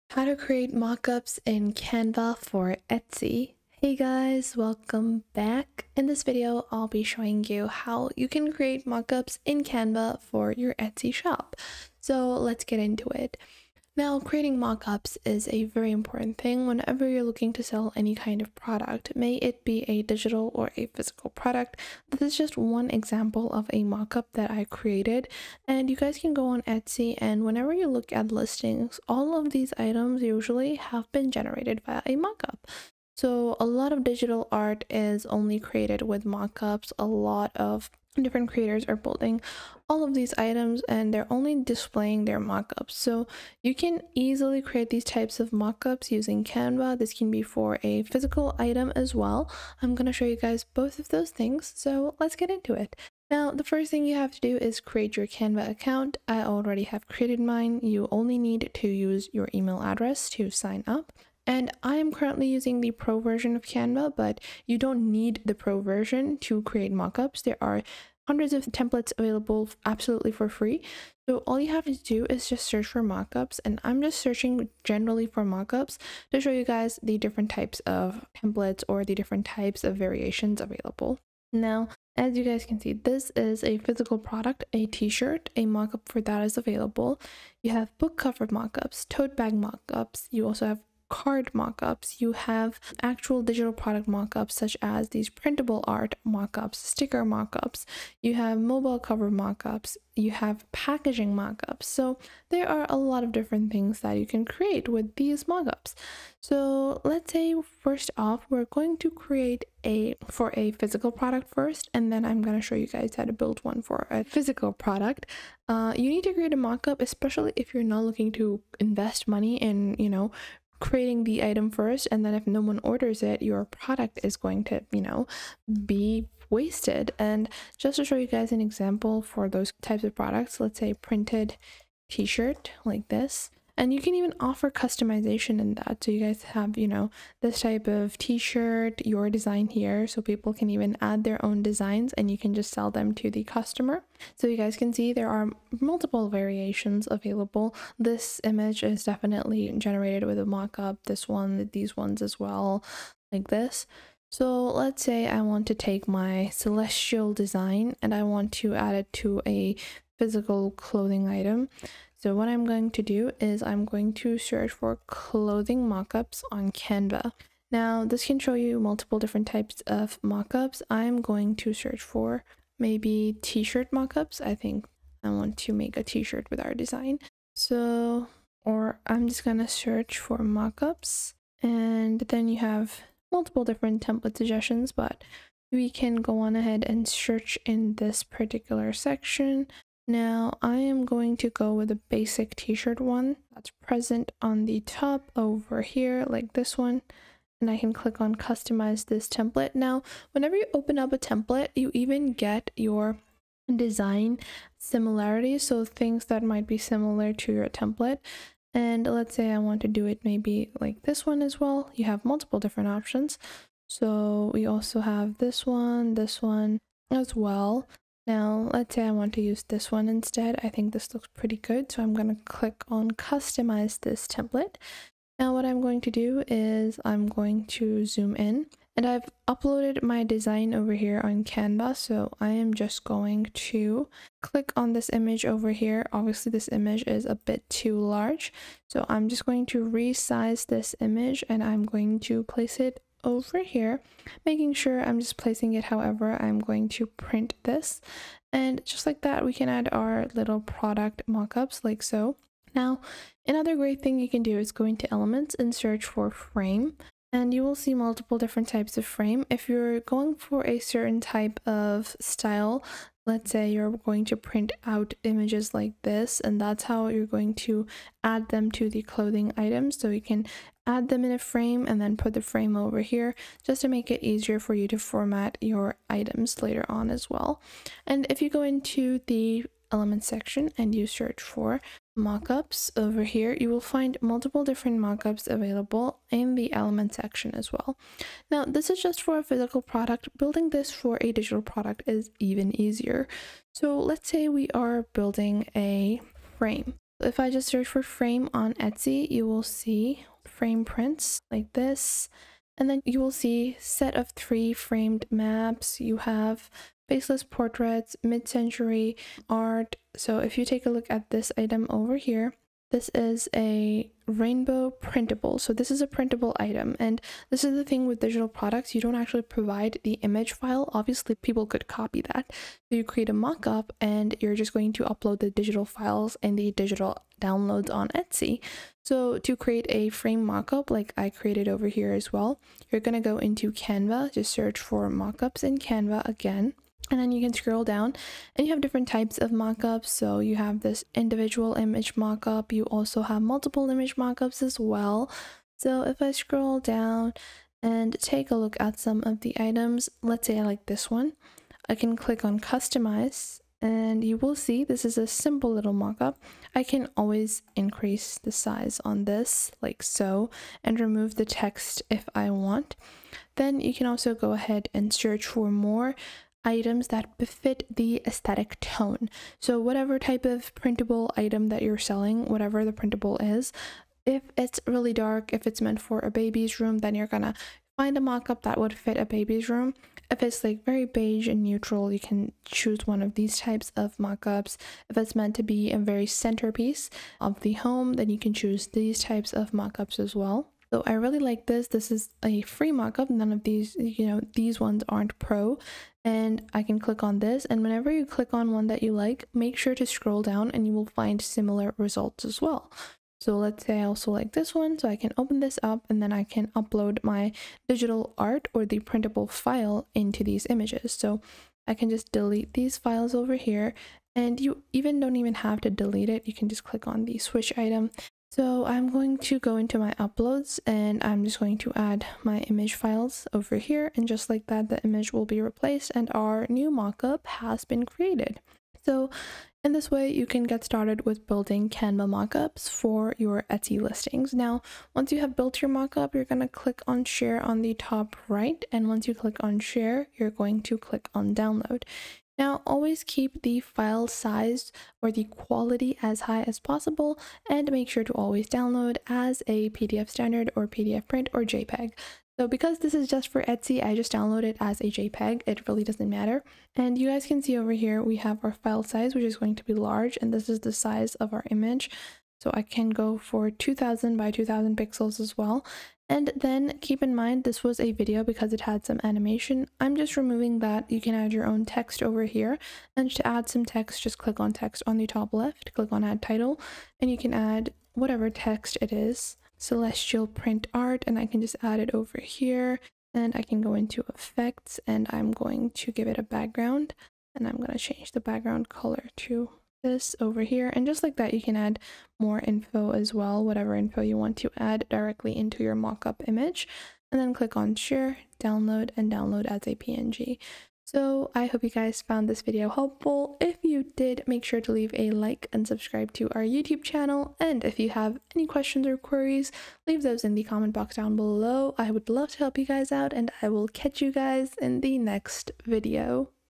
How To Create Mockups In Canva For Etsy (2026) Step by Step
Mockups are an essential aspect of showcasing products on Etsy, as they allow potential customers to visualize the items in use and make informed purchasing decisions. Canva, a popular online graphic design platform, offers an easy and efficient solution for creating mockups for Etsy listings. To begin, select a mockup template from Canva's library or create a custom design.

Next, upload your product image and adjust the placement and size to fit the mockup template. Finally, add any text or design elements to enhance your mockup, and save the final product as a high-quality image. With Canva's user-friendly interface and vast array of design options, creating eye-catching mockups for Etsy has never been easier.